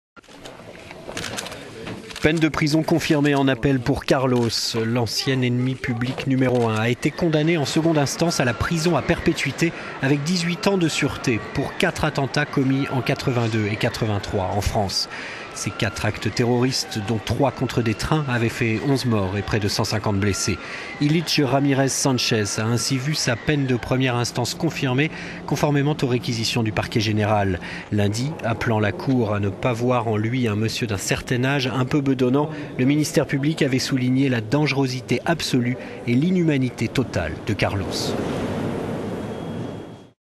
Carlos: la prison à perpétuité confirmée en appel
L'ancien ennemi public numéro Un a été condamné en seconde instance à la prison à...

L'ancien ennemi public numéro Un a été condamné en seconde instance à la prison à perpétuité avec 18 ans de sûreté, pour quatre attentats commis en 1982 et 1983 en France.

Les quatre attentats, dont trois contre des trains, avait fait onze morts et près de 150 blessés.

Ilich Ramirez Sanchez a ainsi vu sa peine de première instance confirmée, conformément aux réquisitions du parquet général.

Lundi, le ministère public avait souligné la "dangerosité absolue" et "l'inhumanité totale" de Carlos.